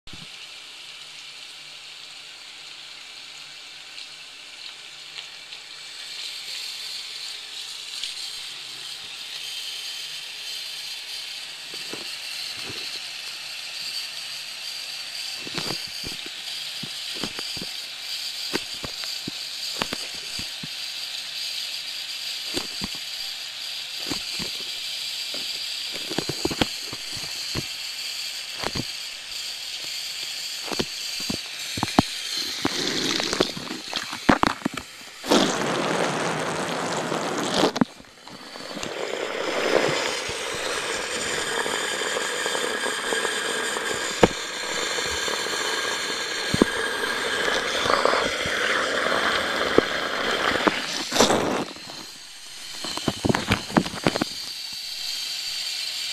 Aquapac FlipMinoHD first test
Testing out the Aquapac that i got today. Put my flipMino HD in it and filled up the sink with water and submerged it while recording. It stayed dry and took video. Plus even in the aquapac case all the buttons worked. Hardest one to get working was power cause it's on the side of the flip, but even that wasn't too hard. Great product! If you want an easy waterproof case for small electronics check out Aquapac, that's a link to the case I bought and are using. They have different sized cases for different devices. Works great!